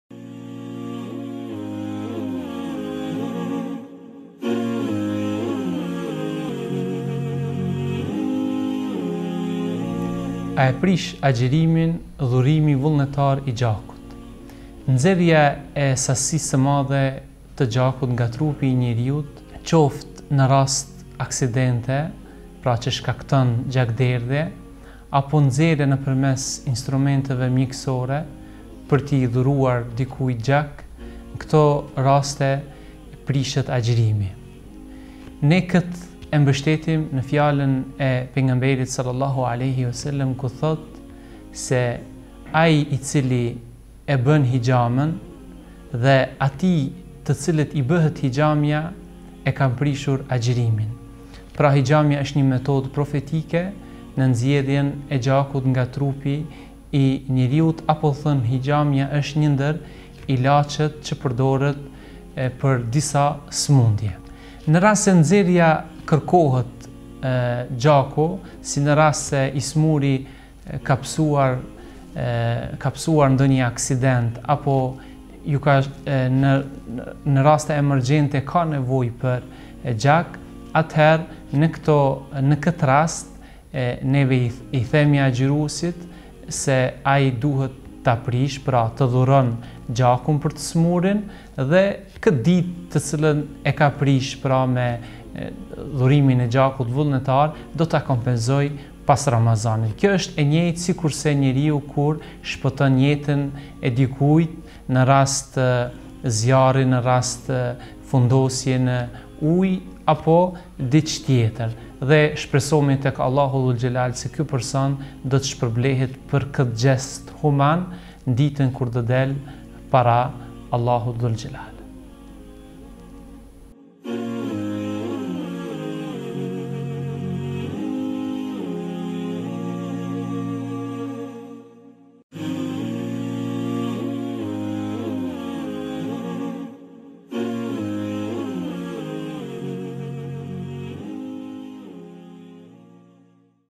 2. A e prish agjërimin dhurimi vullnetar i gjakut?
SERIA "  FETAVA MJEKESORE TE RAMAZANIT  "

AUTOR: HOXHË RASHIT ZYLFIU

KAMERAMANË: SHQIPRIM EMINI&VALON DËRMAKU

MONTAZHI & POST PRODUCTION: MIGJEN CAKA

EKIPI TEKNIKË:

RINOR BEDIJ

ERTON MALIQI

ENIS MAROVCI

REALIZOI: OJQ NJËSHMËRIA, GJILAN

QERSHOR & RAMAZAN, 2014 /1435h